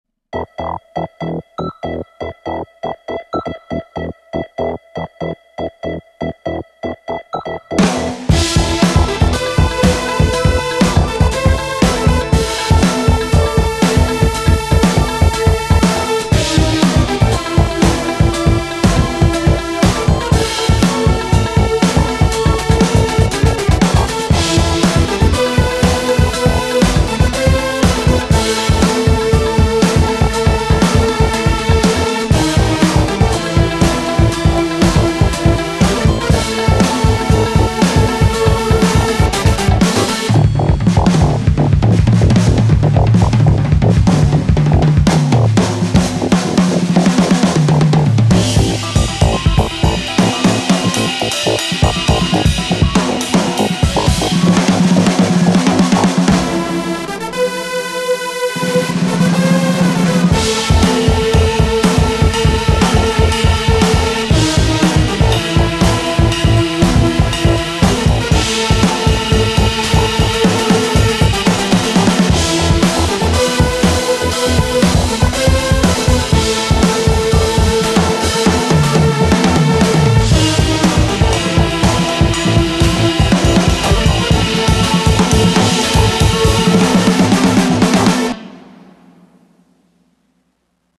INSANE Synth Drum Jam ft. UDO SUPER 6 and Mike Avenaim!
The mini series of original compositions. All synths recorded with the UDO SUPER 6. No triggers or samples used on the Drums.

Composed, Engineered and Mixed by Mike Avenaim.